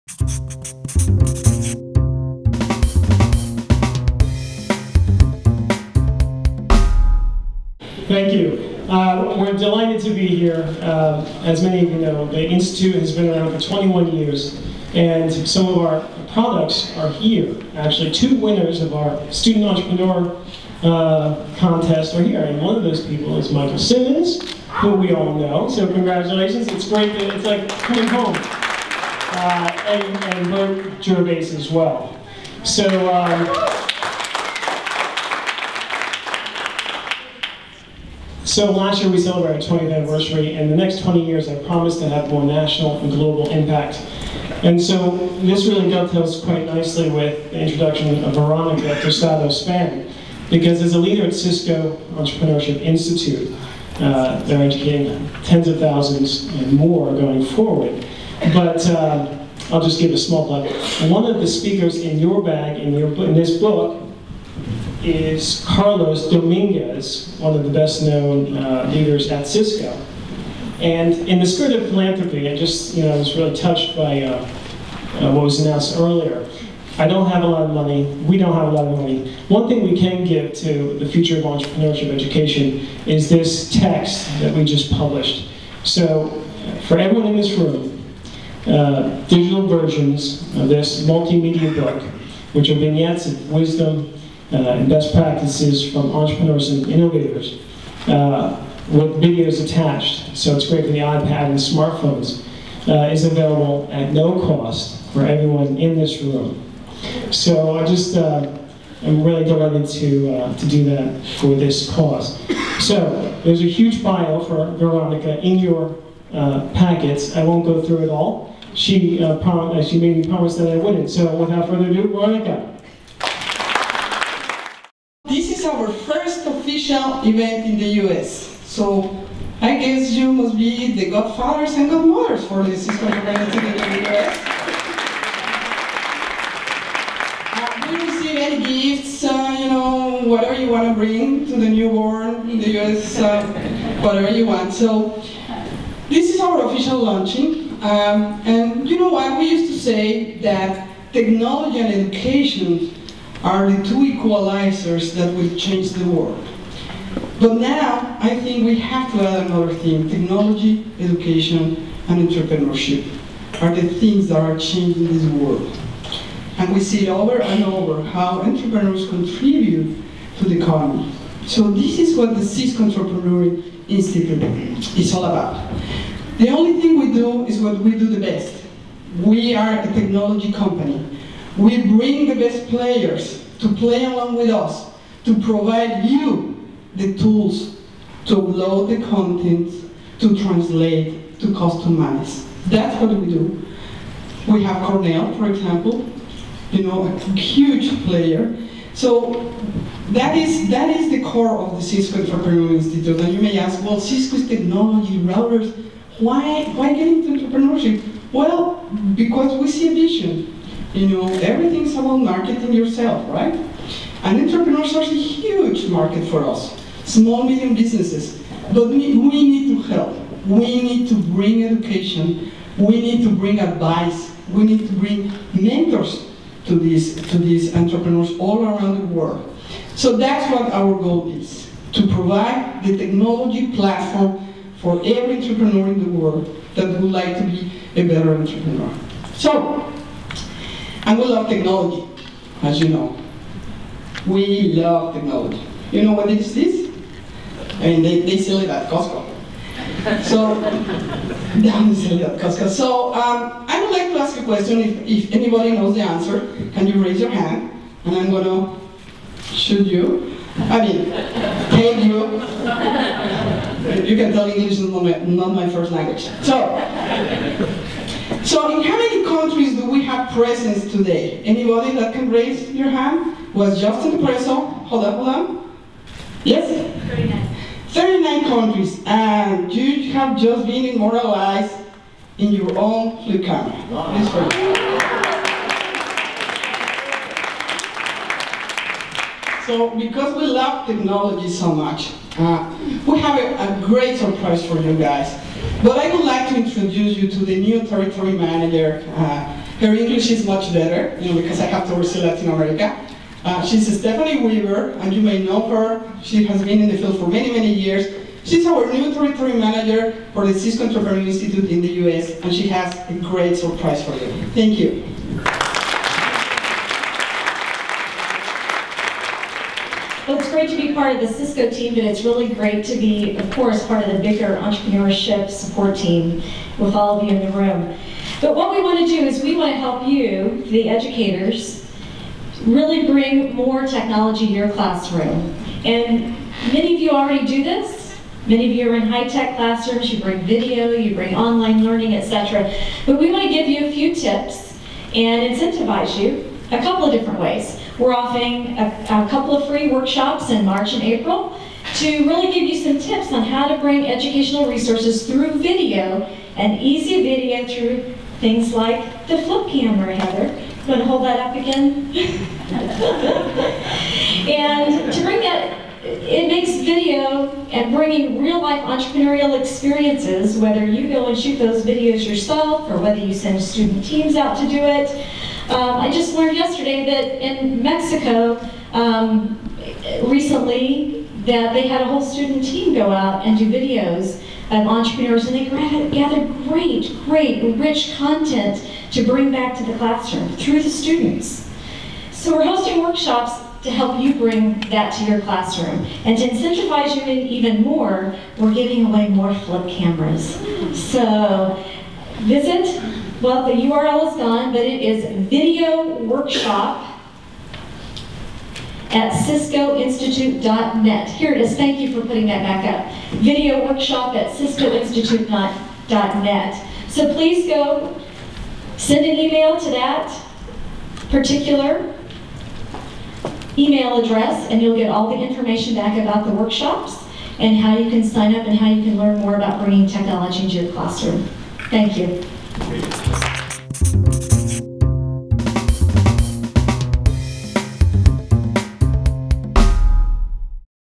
Veronica Tostado-Span at Future of Entrepreneurship Education
Veronica Tostado-Span - Veronica Tostado-Span is Regional Manager at Cisco Entrepreneur Institute, Cisco The Cisco Entrepreneur Institute supports entrepreneurs by working with local government and business organizations to foster the growth and success of small businesses and start-ups. The Institute provides practical business insights for entrepreneurs, facilitates knowledge sharing with local business leaders, and shows participants how to take advantage of technology to speed
business growth.

- The Future of Entrepreneurship Education Summit features short presentations on the future of entrepreneurship education from many of the world's top entrepreneurship leaders (foundations, government, corporations, education, non-profits, and media). The purpose of the presentations is to showcase the most innovative initiatives and lessons learned.